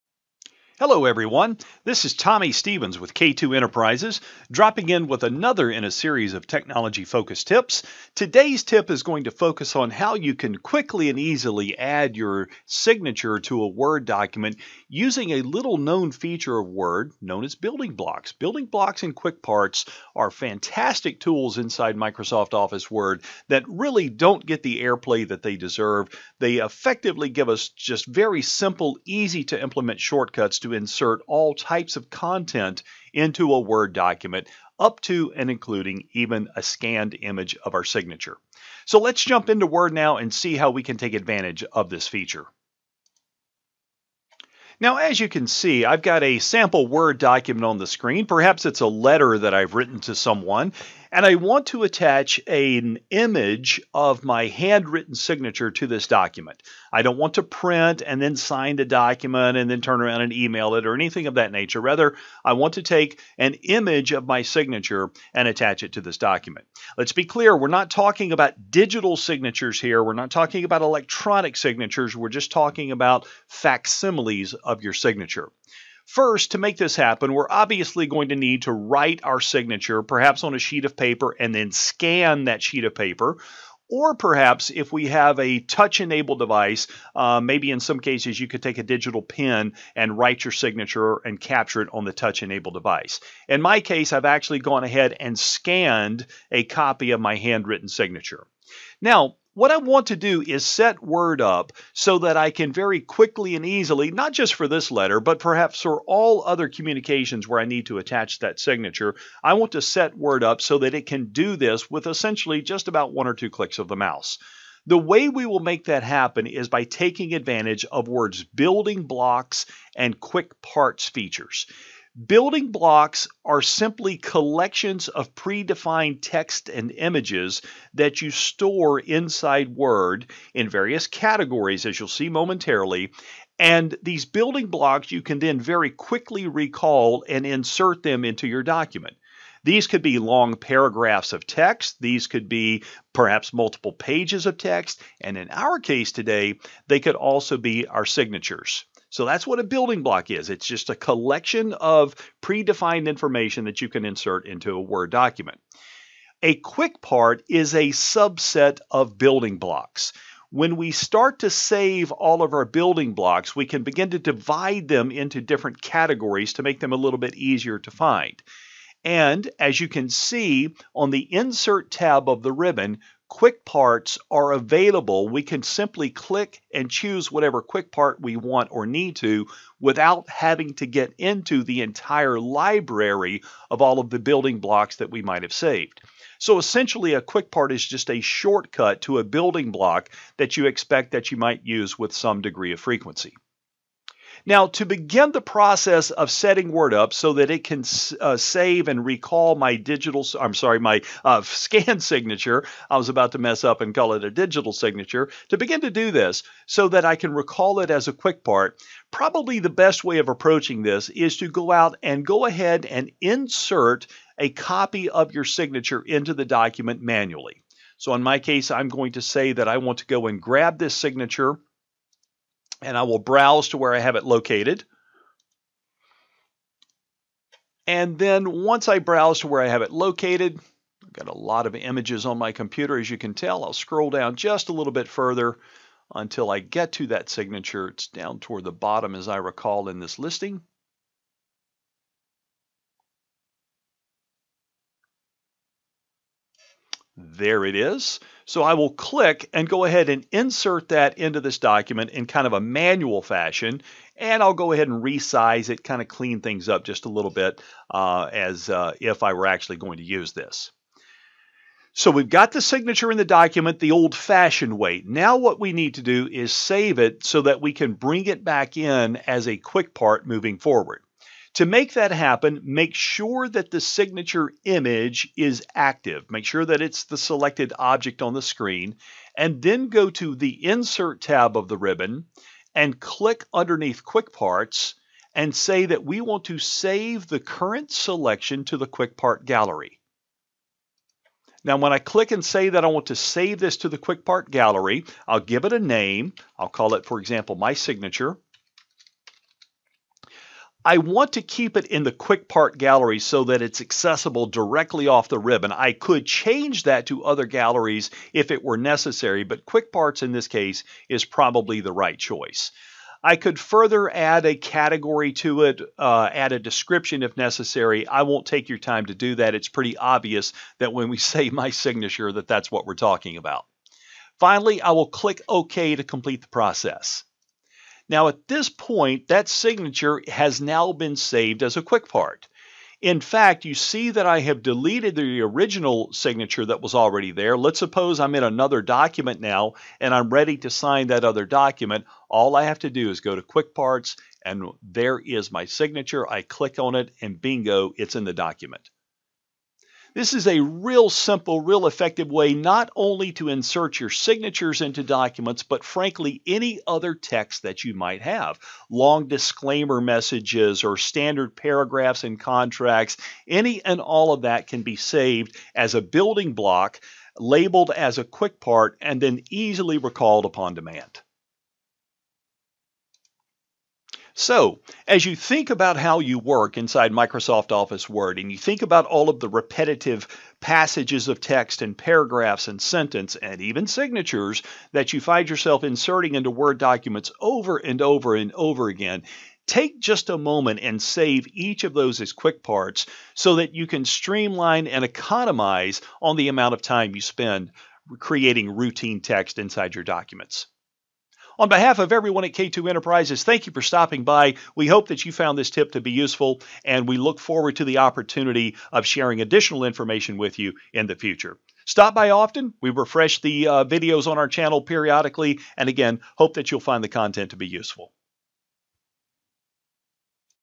Adding Signatures To A Word Document Via Building Blocks
Did you know that you can easily automate the process of adding your signature or any other passages of text to a Word document? Word's Building Blocks and QuickParts features let you store pre-defined content that you can recall on-demand and insert into any document you wish. In this video, you will learn how you can use these tools to streamline the process of inserting your signature and other content into a Word document and save valuable time along the way.